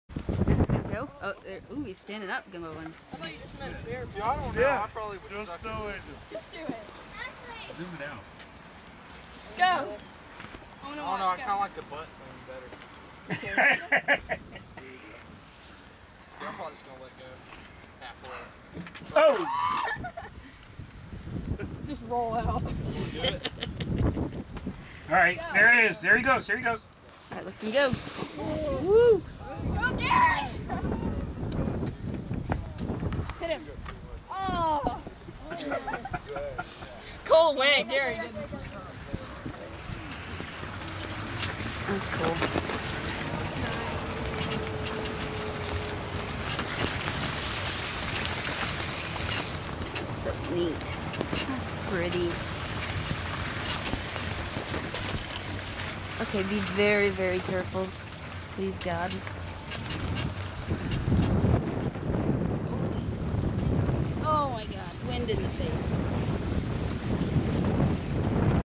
Peachtree City Snow Sled
These are professional sledders (dont do this at home), sledding down Mt. Peachtree in Peachtree City.  People come from miles around just to go sledding and skiing here.  The ski lift was not operational (the blizzard caught the staff off guard) . The sledders had to climb there way back up.  Lift tickets were free as this was the first snow of the season.  Next year we plan on doubling the length of this black diamond run (now 75 feet).